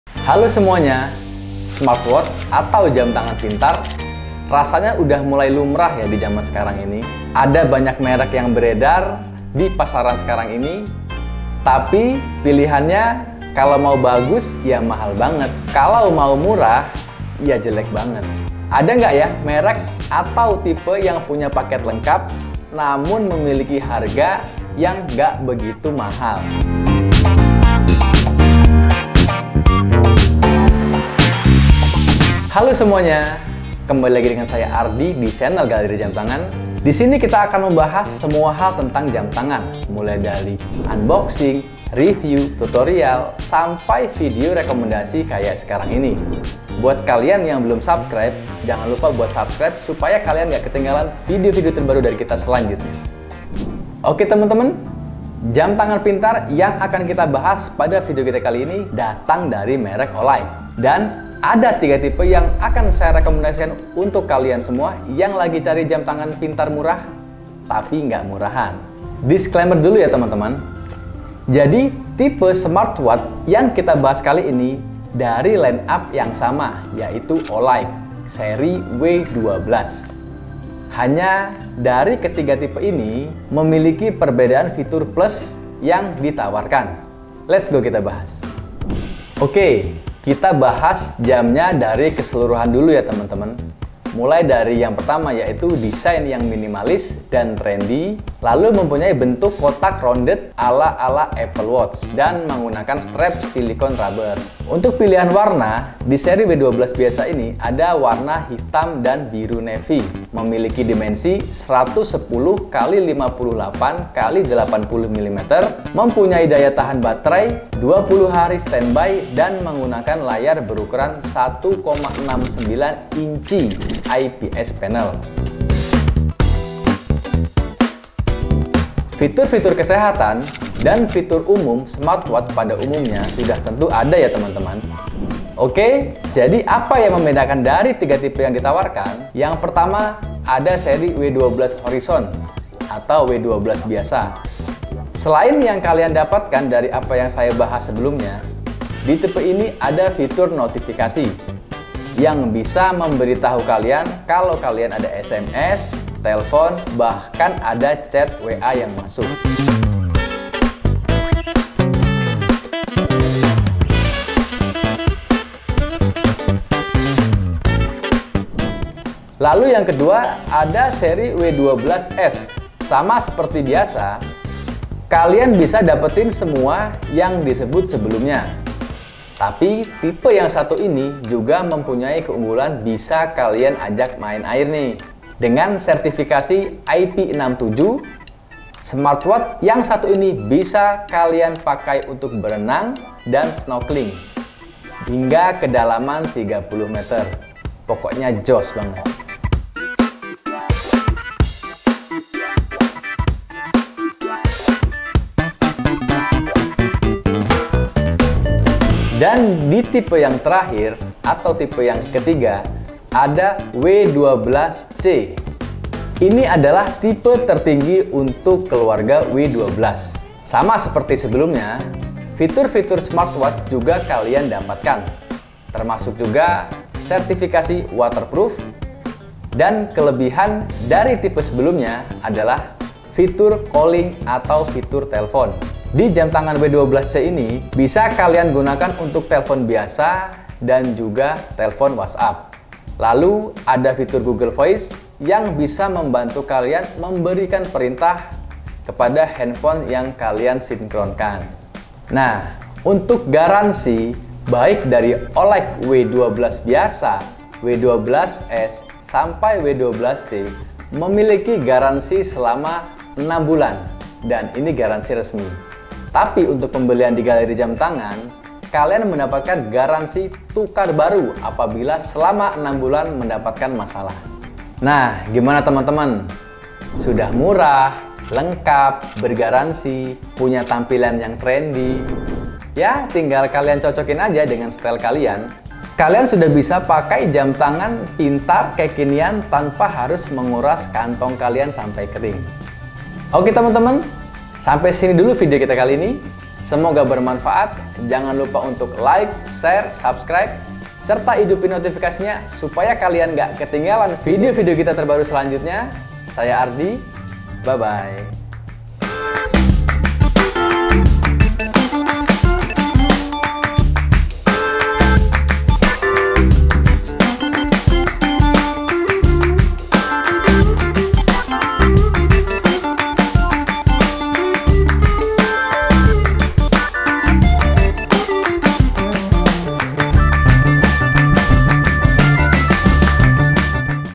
Review Jam Tangan Smartwatch Olike W12 Series I Smartwatch Murah, Fitur Lengkap, Tampilan Kece
Alamat :  Jl. Jenderal Ahmad Yani No.70B, Pelita, Engal, Kota Bandar Lampung, Lampung 35117